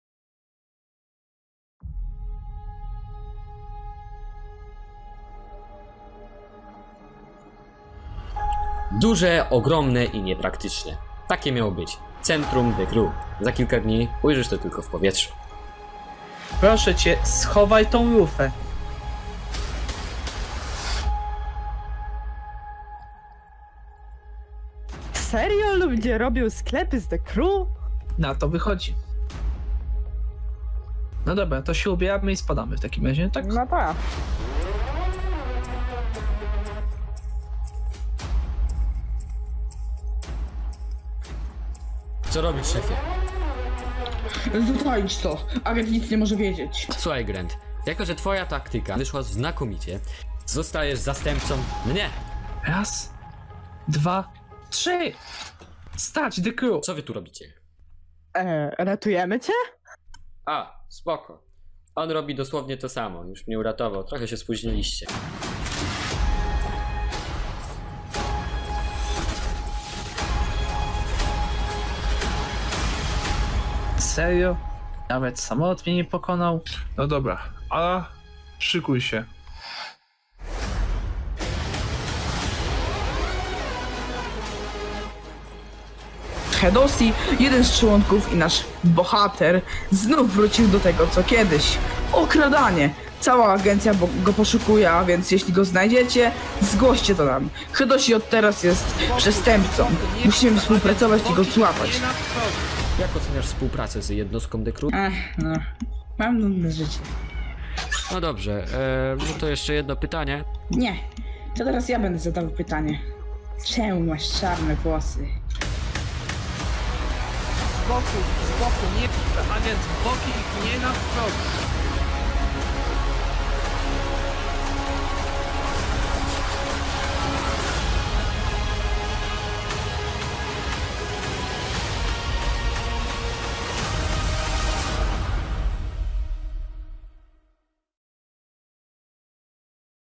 Hedosi Movie - Trailer
💣Hedosi Movie - The Crew szykuje się do startu w powietrze z nową bazą. Całą akcją ma się zająć Filip, ale pojawia się problem. Grend- Osoba, która chce zrobić wszystko, by Hedosiemu się nie powiodło. Co się takiego stało?

👊Spodobał Ci się film? Chcesz więcej? Zostaw łapę i suba!👊

🎤Soundtrack- Jeśli YouTube znalazł... To masz go trochę niżej podanego